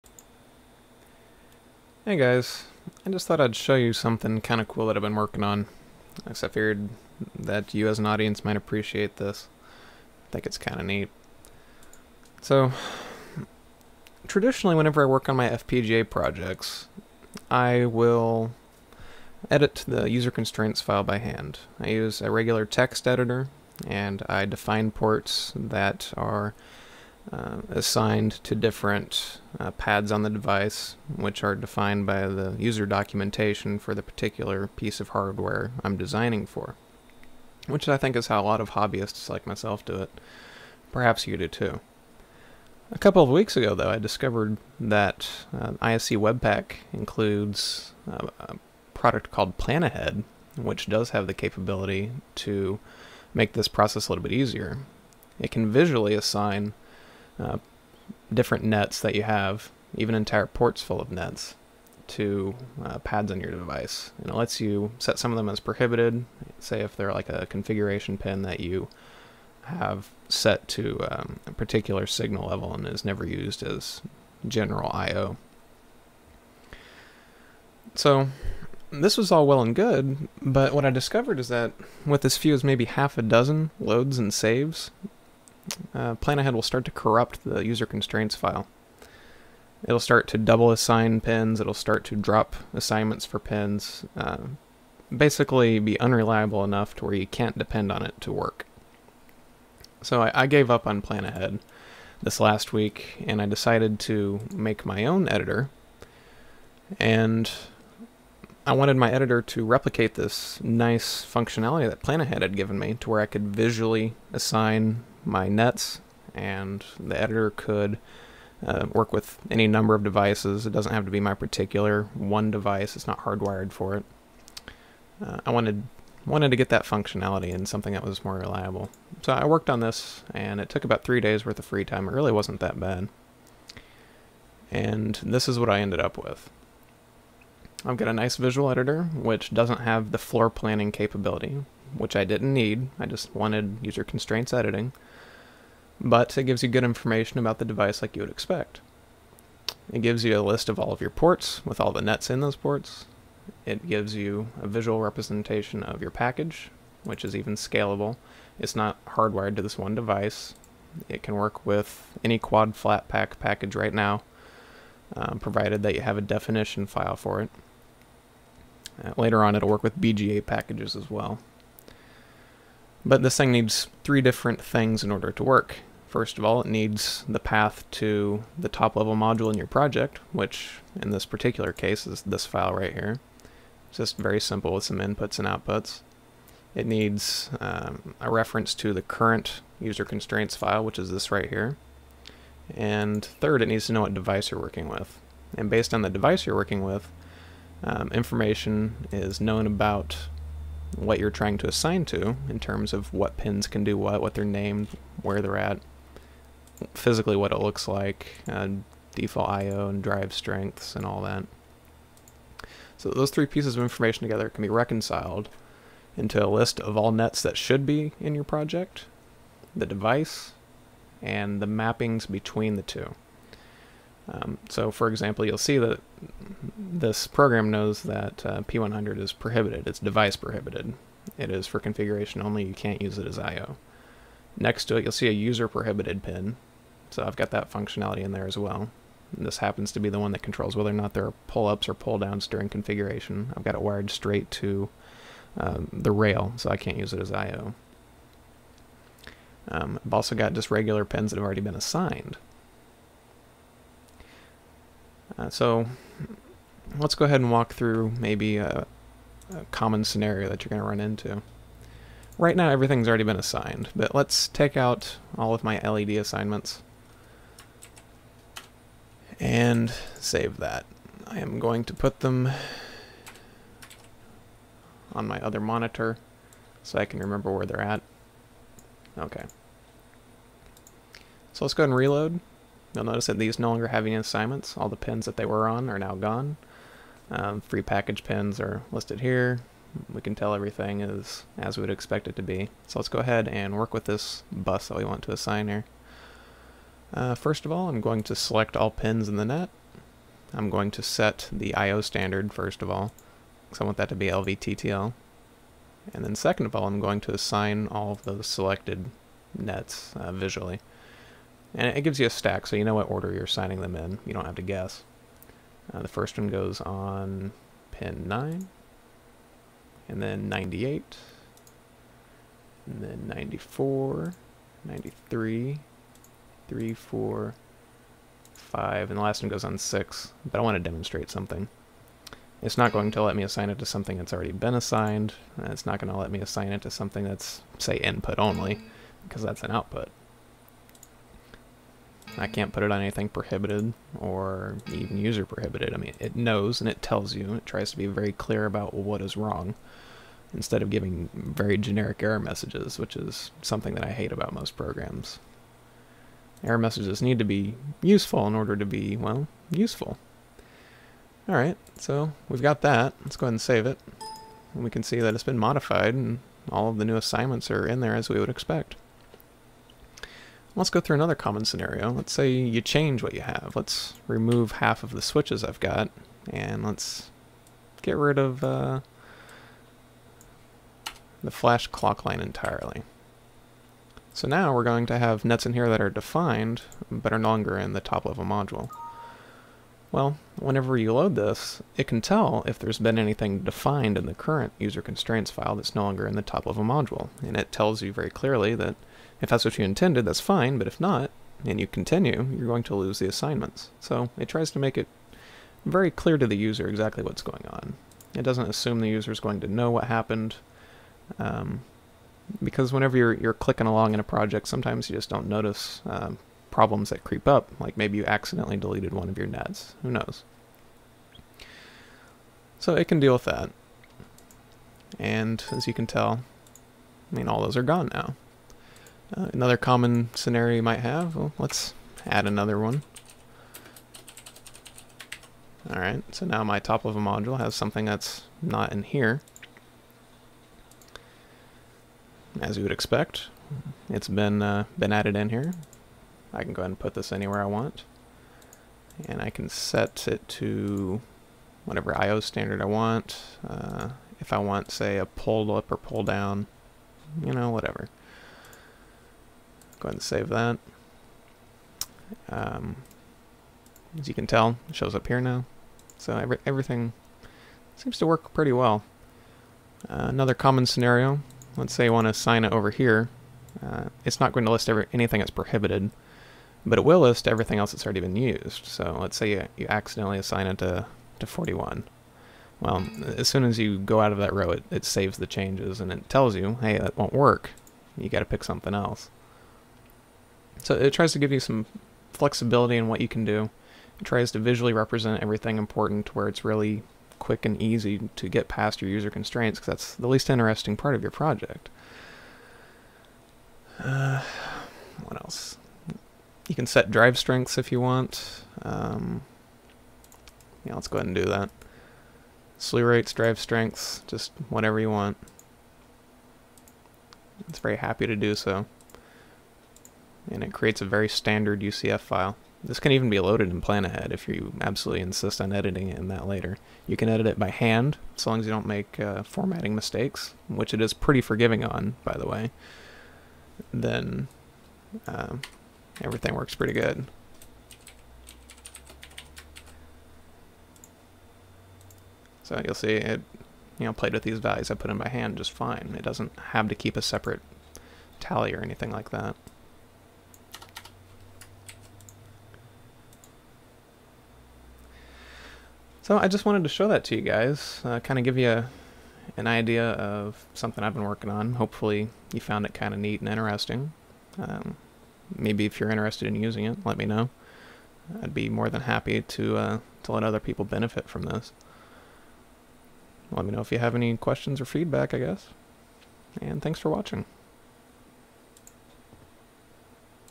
Geoff's FPGA User Constraints Editor
After finding myself hooked on the visual net assignment feature of PlanAhead but endlessly frustrated by PlanAhead's tendency to corrupt UCF files, I took matters into my own hands and created a visual UCF editor.  The process took a few days' free time, and I implemented some improvements over what PlanAhead offers.  This video is a demo outlining what I came up with.